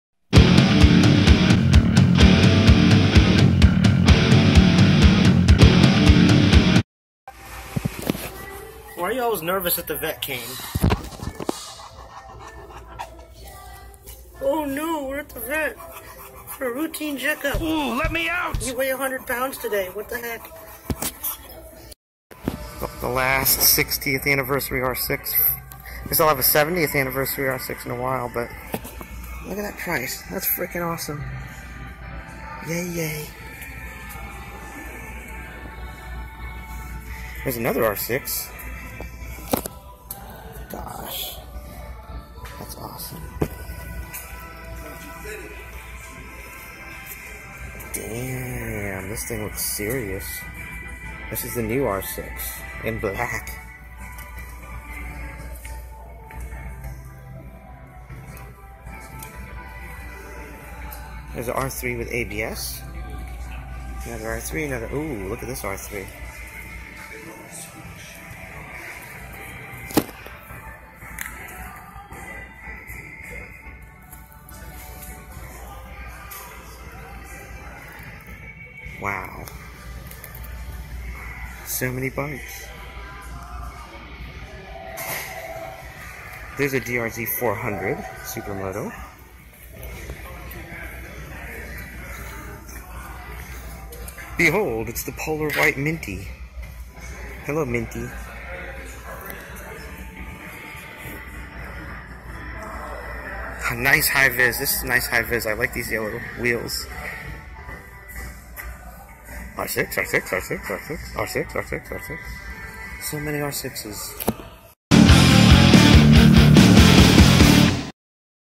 Soo many R6s!!! Deez r a few of me favorite tings mon!!
A small vid of me bumbling around my local moto dealer! But first my heavy baby boy!